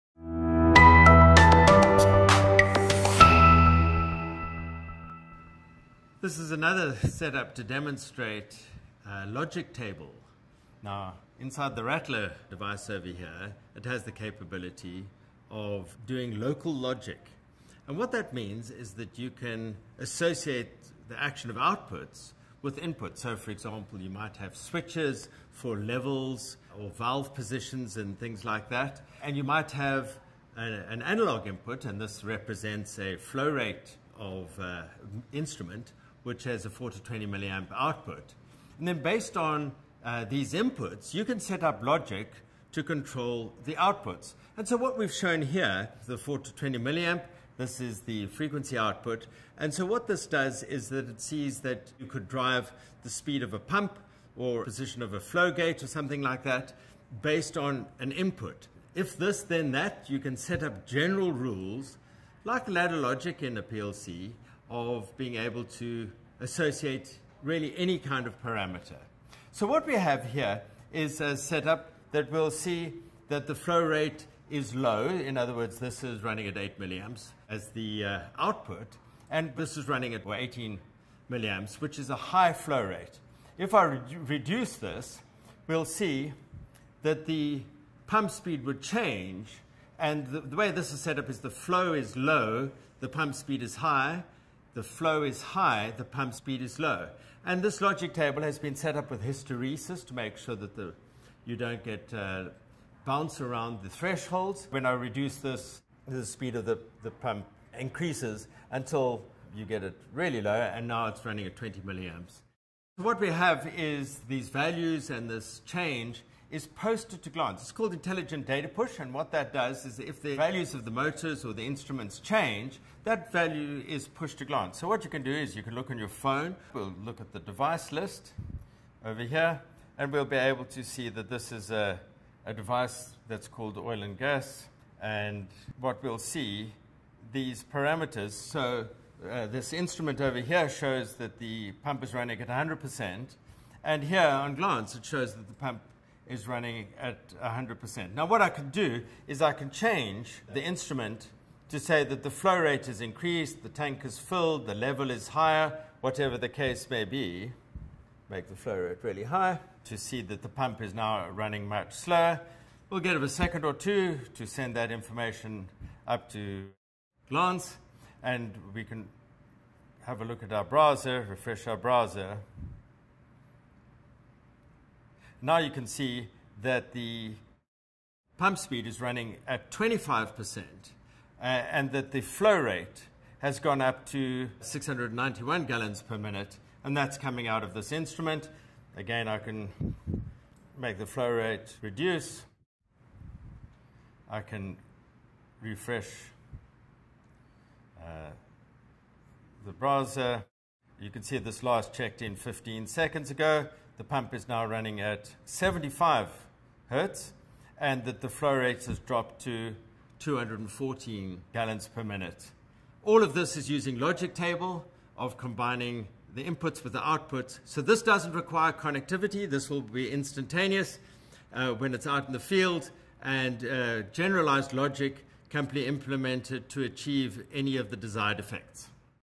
RATTLER Logic Tables Demo
Remotely control your industrial devices and set up logic tables to ensure the right controls and the right time.